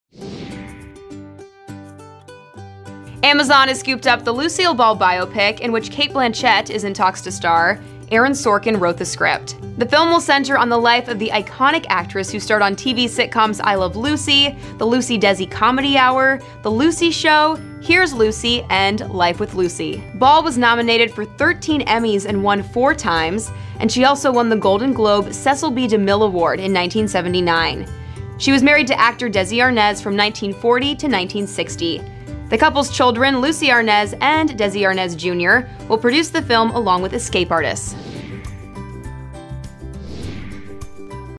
Lucille Ball Biopic Starring Cate Blanchett Lands at Amazon | THR News Flash
Amazon has scooped up the rights to the Lucille Ball biopic that has a script by Aaron Sorkin. Cate Blanchett is in talks to star.

The film will center on the life of the iconic actress, who starred on TV sitcoms I Love Lucy, The Lucy–Desi Comedy Hour, The Lucy Show, Here's Lucy and Life with Lucy. She was nominated for 13 Emmys and won four times, and she also won the Golden Globe Cecil B. DeMille Award in 1979. She was married to actor Desi Arnaz from 1940 to 1960.